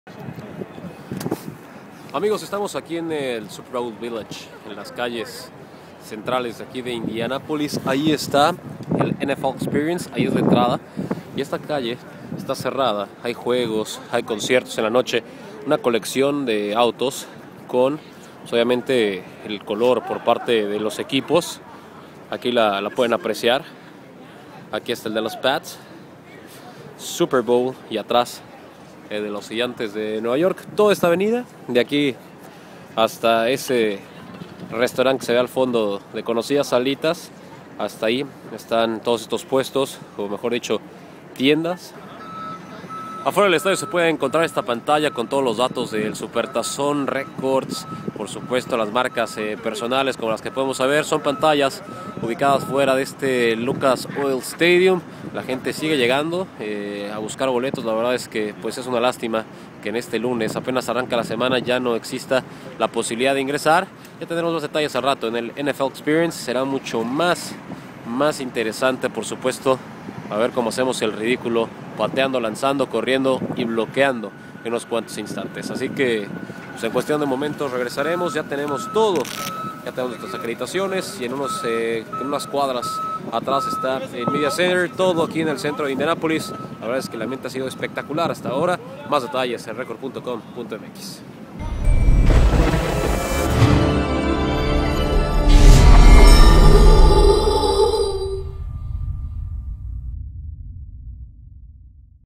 Indianápolis se calienta
Disfruta de los reportes desde Indianápolis, de nuestro enviado Rodrigo Arana, quien te trae todos los detalles de lo que acontece en torno al Super Bowl XLVI durante toda la semana.